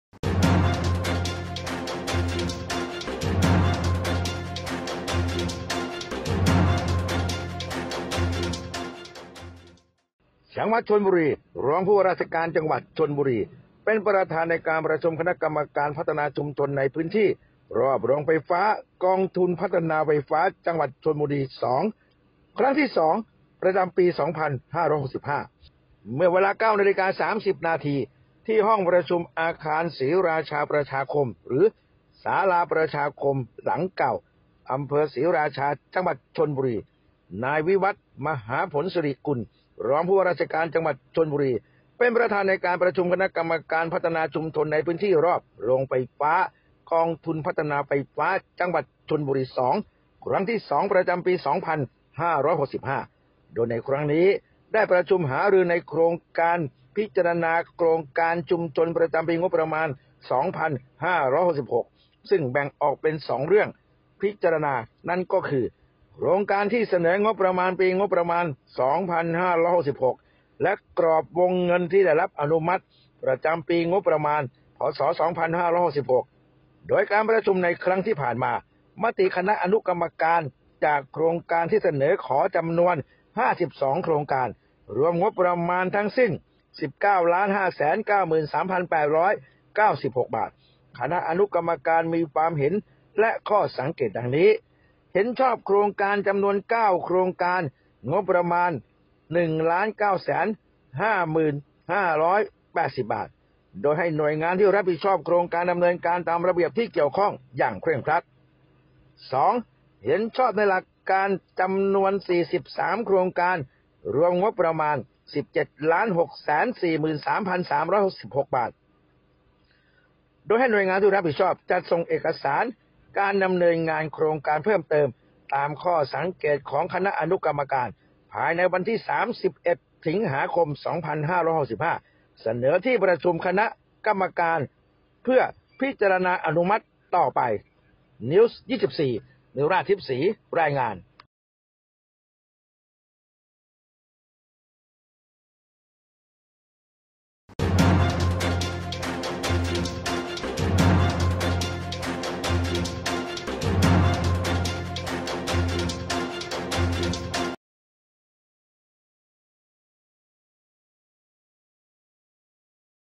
จังหวัดชลบุรี รองผู้ว่าราชการจังหวัดชลบุรี ประชุมคณะกรรมการพัฒนาชุมชนในพื้นที่รอบโรงไฟฟ้ากองทุน
จังหวัดชลบุรี  รองผู้ว่าราชการจังหวัดชลบุรี  ประชุมคณะกรรมการพัฒนาชุมชนในพื้นที่รอบโรงไฟฟ้ากองทุนพัฒนาไฟฟ้าจังหวัดชลบุรี 2 ครั้งที่ 2
ผู้ประกาศ  รองผู้ว่าราชการจังหวัดชลบุรี  เป็นประธานในการประชุมคณะกรรมการพัฒนาชุมชนในพื้นที่รอบโรงไฟฟ้ากองทุนพัฒนาไฟฟ้าจังหวัดชลบุรี 2 ครั้งที่ 2/2565
เทปข่าว      5 กันยายน 2565 เวลา 9.30 น. ที่ ห้องประชุมอาคารศรีราชาประชาคม (ศาลาประชาคมหลังเก่า)อำเภอศรีราชา จังหวัดชลบุรี  นายวิวัฒน์  มหาผลศิริกุล รองผู้ว่าราชการจังหวัดชลบุรี  เป็นประธานในการประชุมคณะกรรมการพัฒนาชุมชนในพื้นที่รอบโรงไฟฟ้ากองทุนพัฒนาไฟฟ้าจังหวัดชลบุรี 2 ครั้งที่ 2/2565  โดยครั้งนี้ ได้ประชุมหารือในการพิจารณาโครงการชุมชนประจำปีงบประมาณ พ.ศ. 2566 ซึ่งแบ่งออกเป็น  2 เรื่องพิจารณา  คือ โครงการที่เสนอขอประจำปีงบประมาณ พ.ศ. 2566  และ กรอบวงเงินที่ได้รับอนุมัติประจำปีงบประมาณ พ.ศ.2566  โดยการประชุมในครั้งที่ผ่านมา มติคณะอนุกรรมการฯ จากโครงการที่เสนอขอจำนวน 52 โครงการ รวมงบประมาณทั้งสิ้น 19,593,896.00 บาท
คณะอนุกรรมการฯมีความเห็นและข้อสังเกตดังนี้
1.เห็นชอบโครงการจำนวน 9 โครงการ งุบประมาณ 1,950,580.00 บาท
โดยให้หน่วยงานที่รับผิดชอบโครงการ ดำเนินการตามระเบียบที่เกี่ยวข้องอย่างเคร่งครัด
2.เห็นชอบในหลักการจำนวน 43 โครงการ รวมงบประมาณ 17,643, 316.00 บาท
โดยให้หน่วยงานที่รับผิดชอบจัดส่งเอกสารการดำเนินงานโครงการเพิ่มเติมตามข้อสังเกตของคณะอนุฯ
ภายในวันที่ 31 สิงหาคม 2565 เสนอที่ประชุมคณะกรรมการ คพรฟ. เพื่อพิจารณาอนุมัติต่อไป